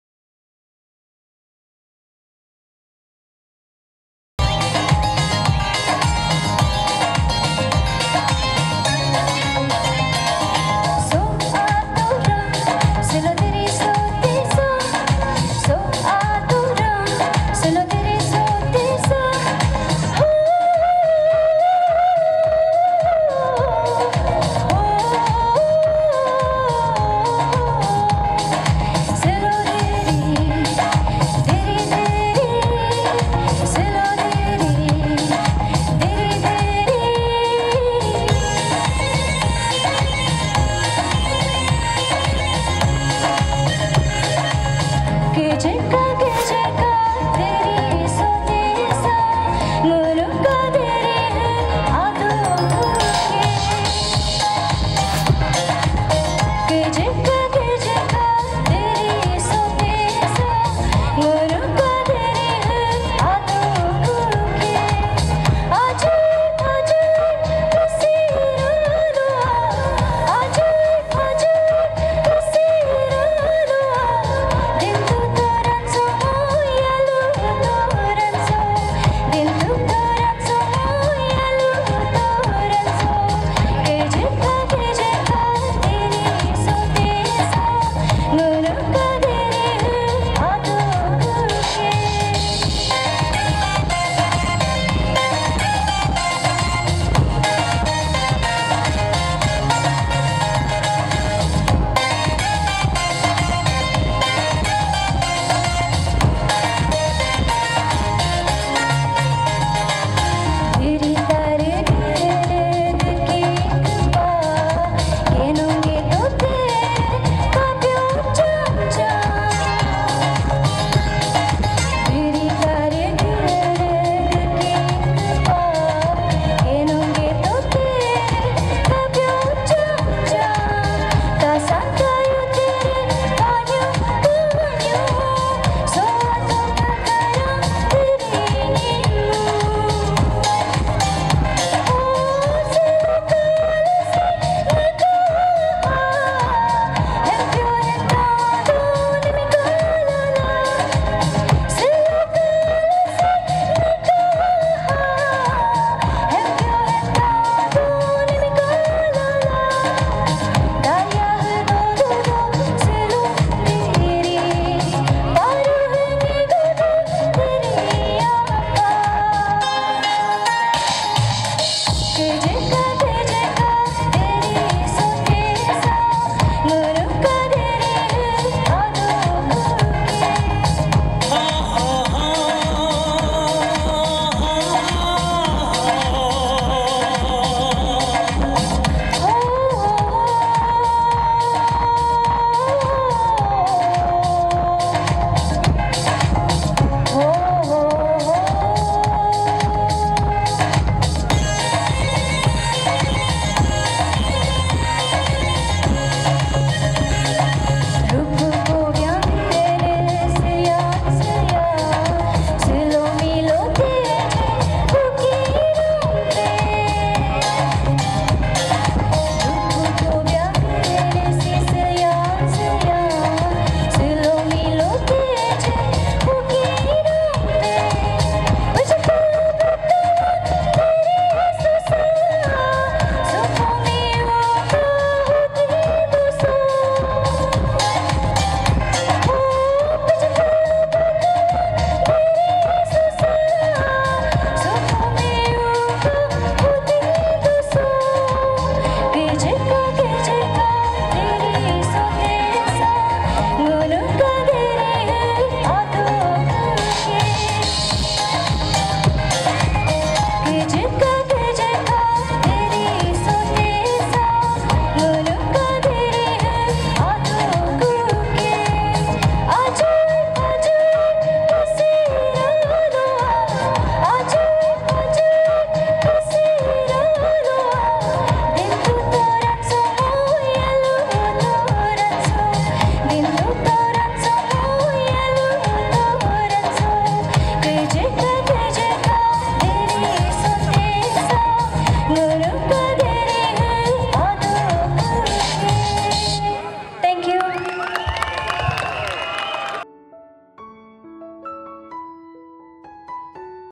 Tamo Annu, voice of Dree 2023
Miss Tamo Annu, participants of Voice of Dree, Season IV 2023 organised by Central Dree Festival Committee-2023